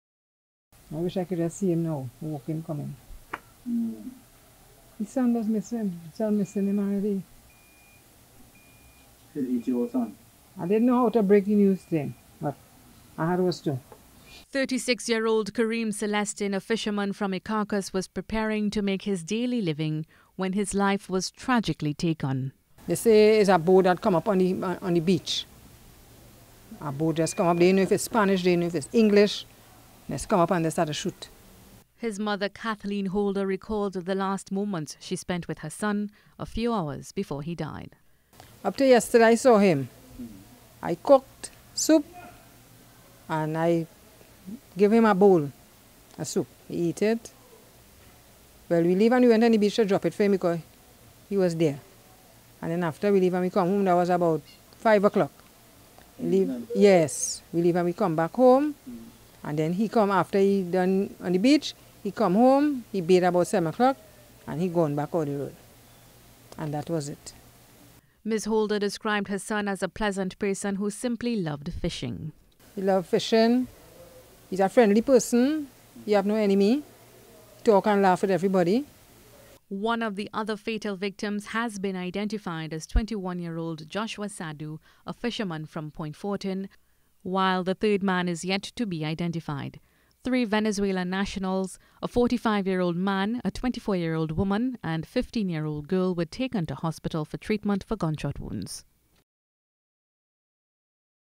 Three Men Shot Dead At Icacos Beach
Three people are dead and three others wounded after gunmen opened fire at Icacos beach on Thursday morning.

More in this report.